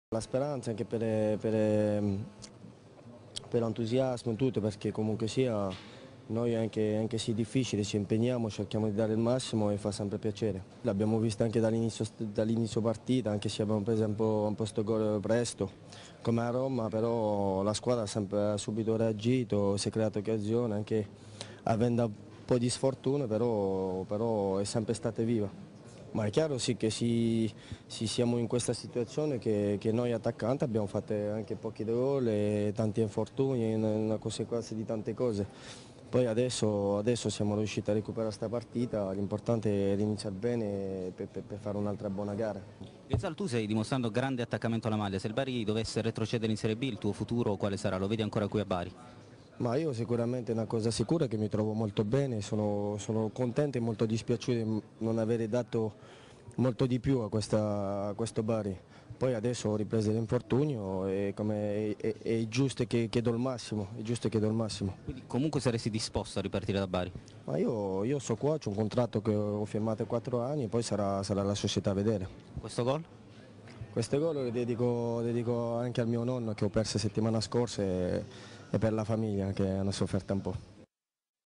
Intervista a Abdelkader Ghezzal
Intervista a Abdelkader Ghezzal(Attaccante del Bari), intervistato in sala stampa allo Stadio San Nicola di Bari, dopo la partita Bari-Fiorentina 1-1.